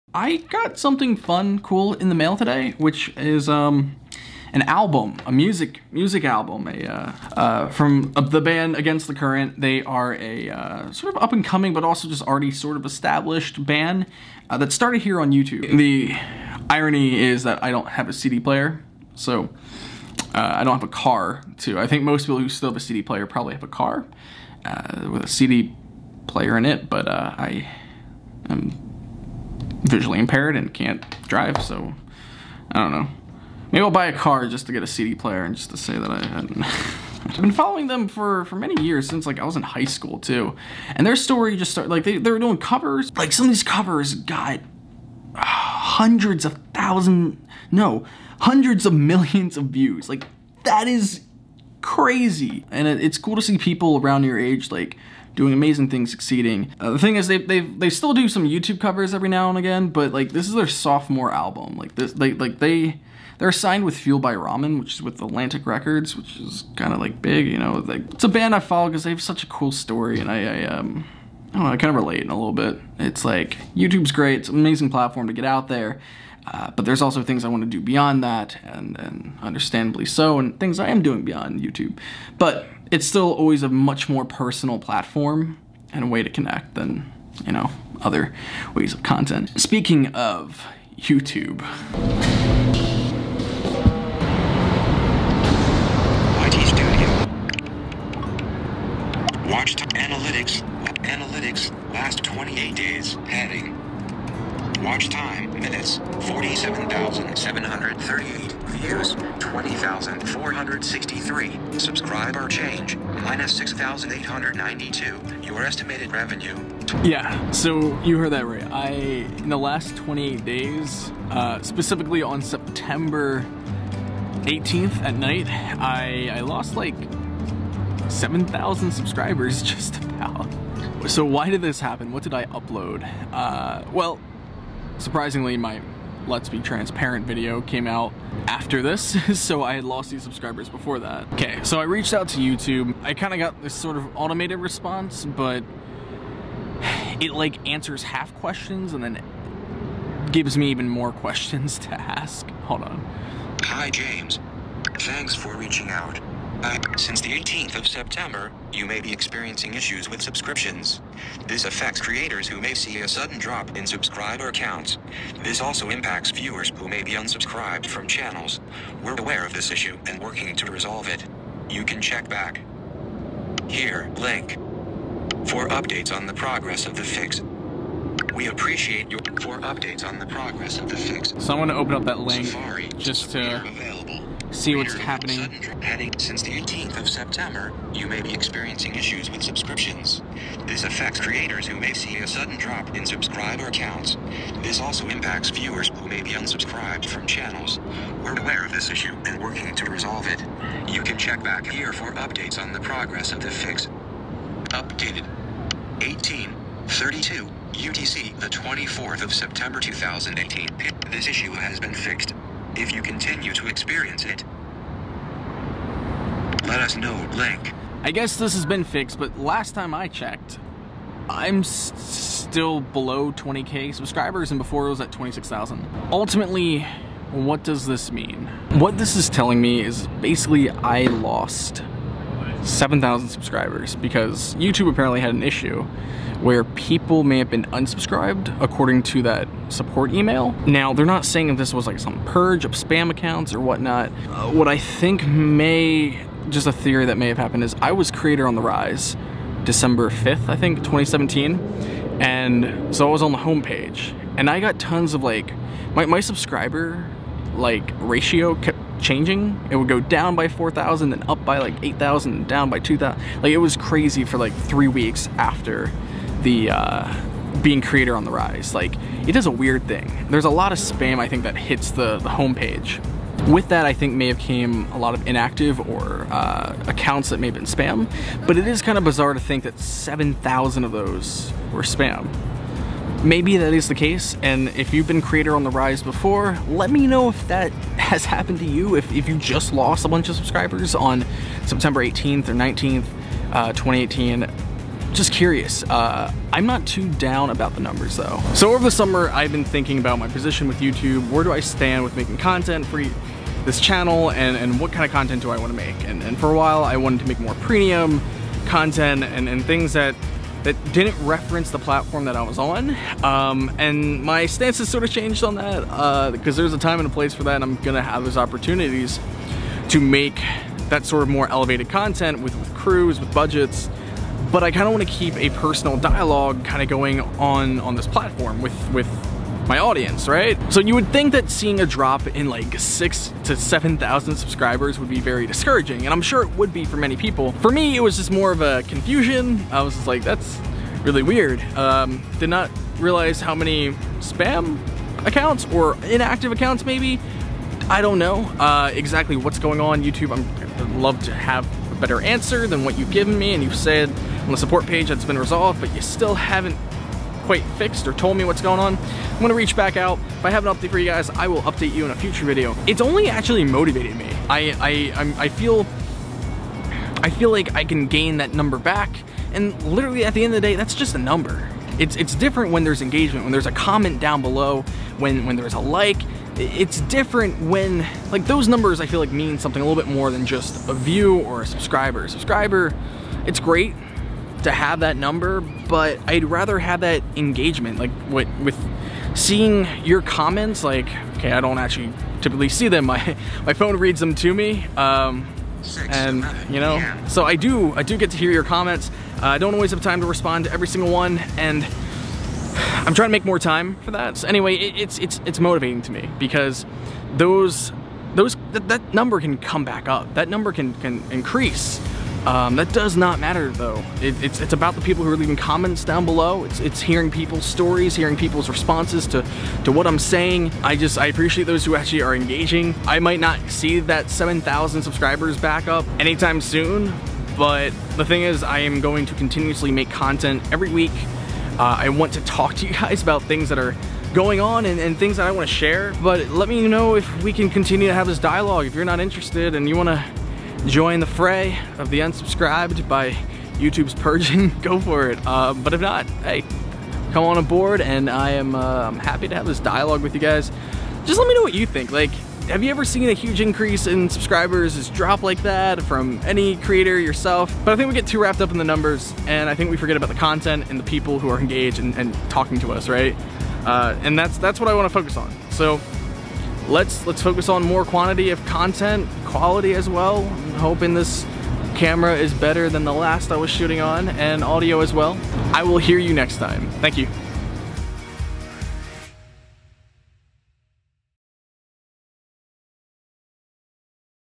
I Lost 7,000 Subscribers Overnight!
So I noticed a huge decrease in subscribers the other day... I want to address why I believe so many people unsubscribed.

Special thanks for supporting my work!
Caroline C.
Denielle E.